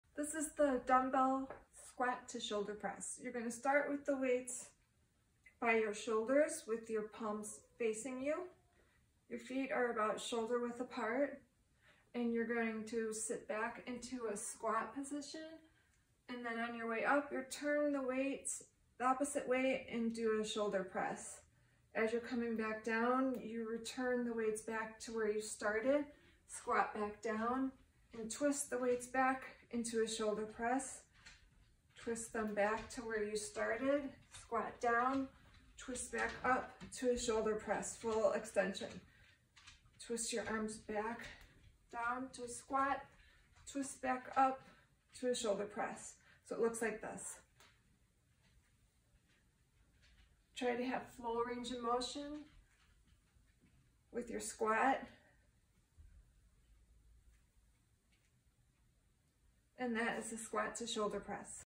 Dumbbell Squat to Shoulder Press (Beginner)
Hold light dumbbells in front of your shoulders, palms facing you.

As you drive up reverse your grip to palms out.

Push the weights up above your head; driving through your heels.